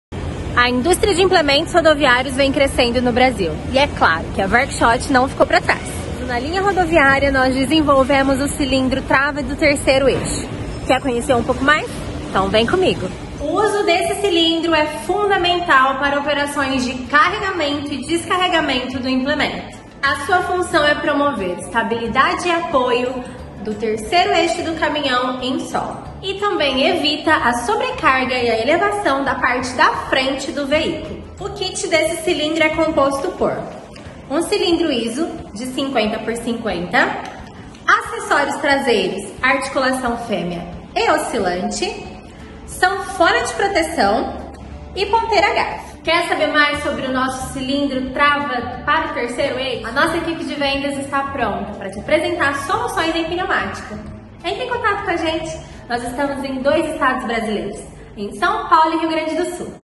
WERK-SCHOTT - Cilindro Trava do 3º Eixo
Cilindro montado com acessórios, destinado a equipar veículos da Linha Rodoviária, com a finalidade de Garantir o Travamento do 3º eixo. o cilindro é fundamental para garantir as operações de carregamento e descarregamento do implemento, permitindo a estabilidade e apoio do 3º eixo em solo, evitando a sobrecarga e elevação da frente do veículo. Desenvolvido para a função específica para a Linha Rodoviária, é um cilindro normalizado ISO Ø50 X 50mm, equipado com acessórios de fixação traseira – Articulações Traseira Macho / Fêmea, fixação dianteira – Ponteira Garfo, além de Sanfona de Proteção e Conexões Instantâneas.
Peça a sua cotação pela nossa Central de Vendas no fone/whats (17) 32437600 ou através do